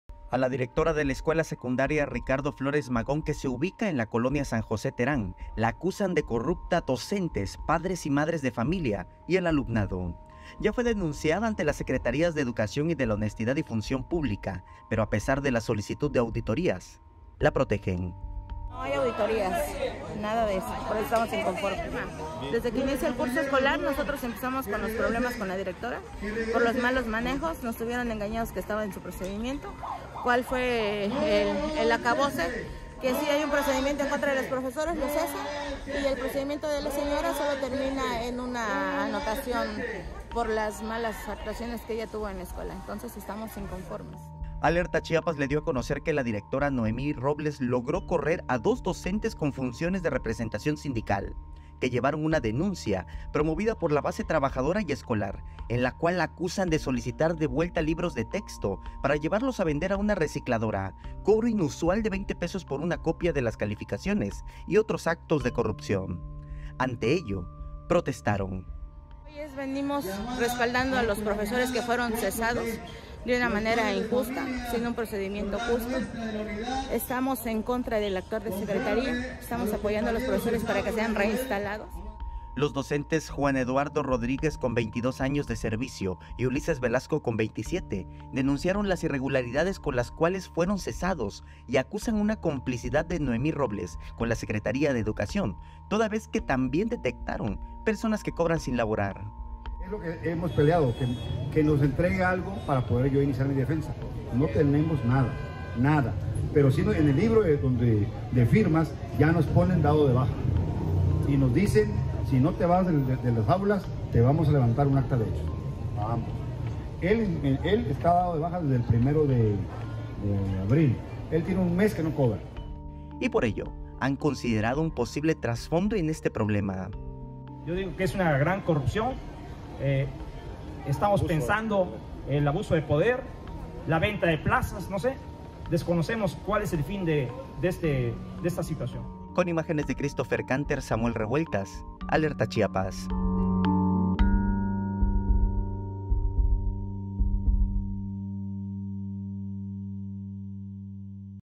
No hay auditoría para directora acusada de vender libros de texto gratuitos
A la directora de la Escuela Secundaria Ricardo Flores Magón que se ubica en la colonia San José Terán la acusan de corrupta docentes, padres y madres de familia y el alumnado.